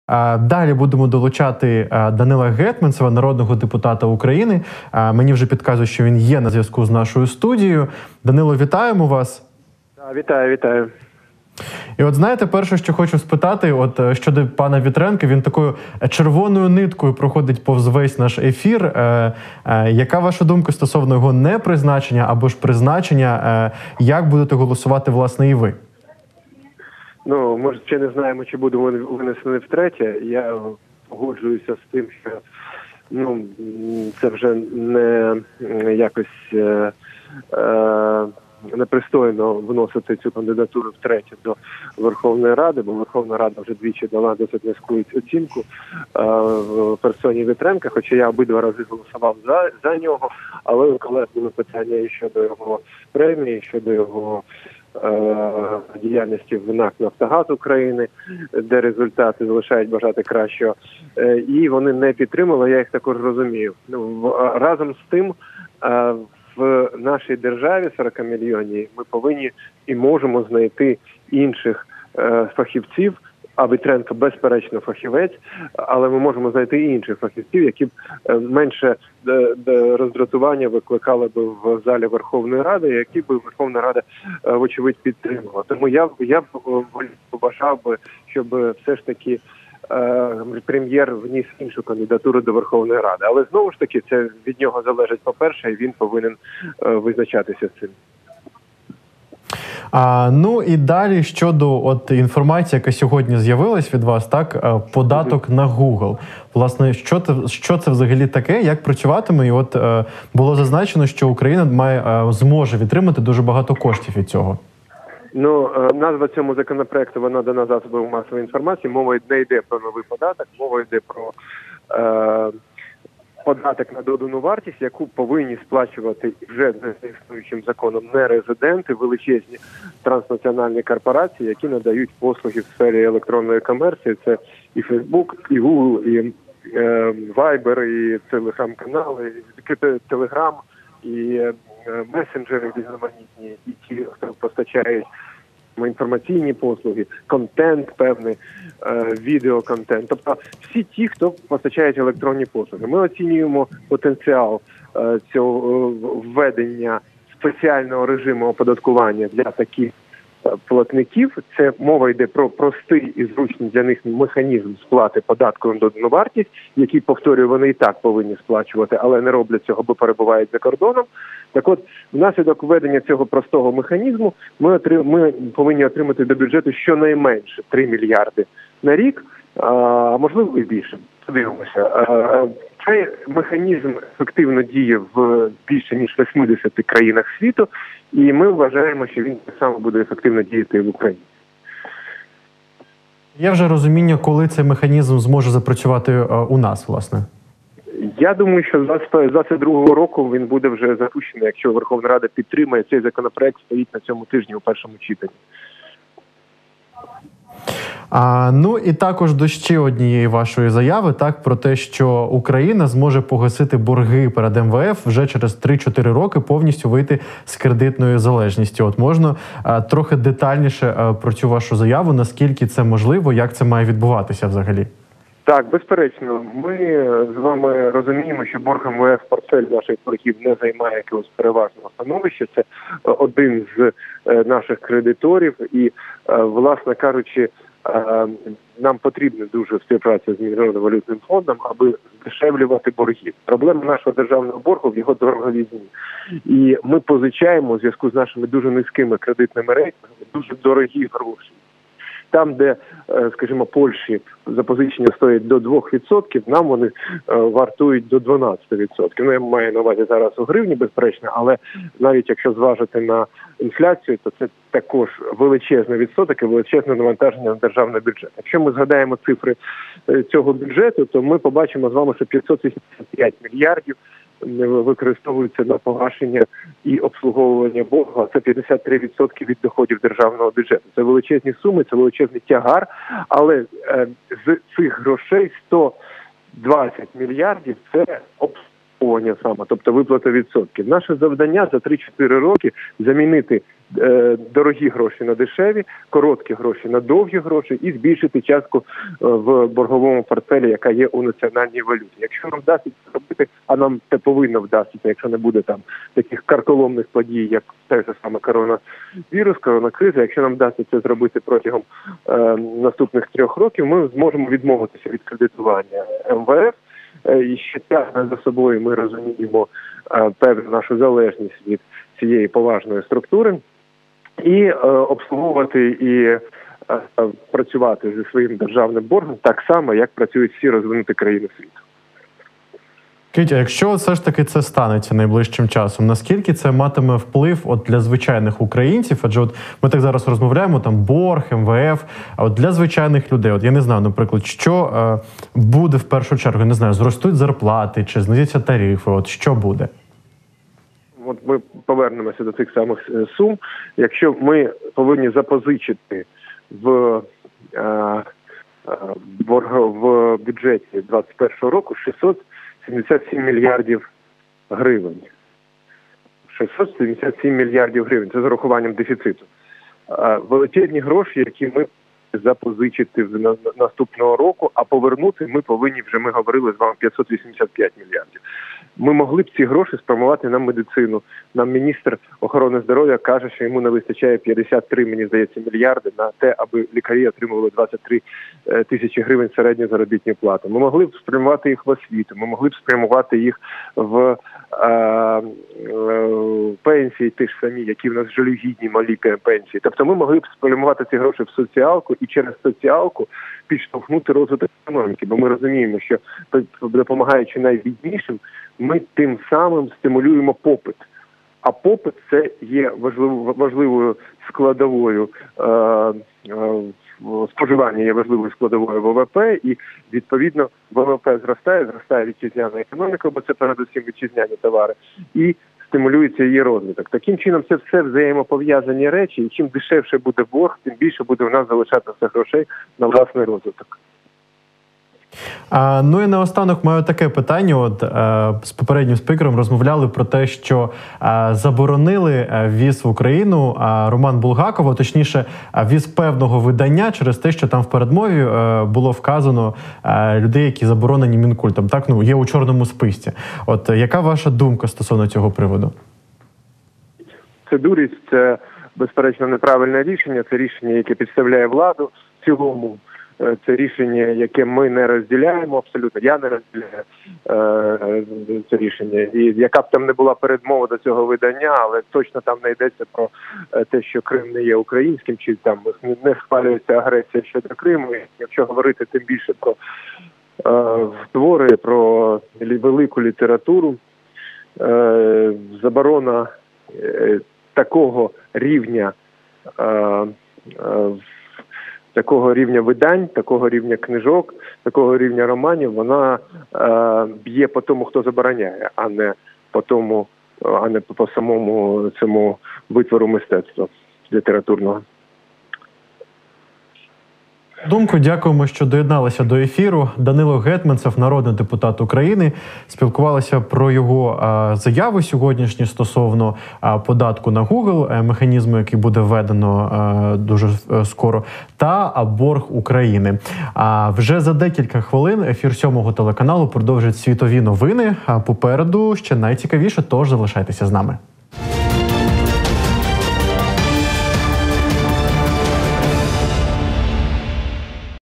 Чи зможе Україна погасити свої заборгованості перед кредиторами у 2021 році?
Голова фінансового комітету Ради впевнений, що Україна зможе погасити борги МВФ і вже через 3–4 роки вийти з кредитної залежності
Данило Гетманцев заявив, що Україна має достатній фінансовий потенціал, щоб погасити свої заборгованості в 2021 році. І мова йде не тільки про виплати МВФ, але і іншим кредиторам.

на прямому зв'язку:
Данило Гетманцев, народний депутат України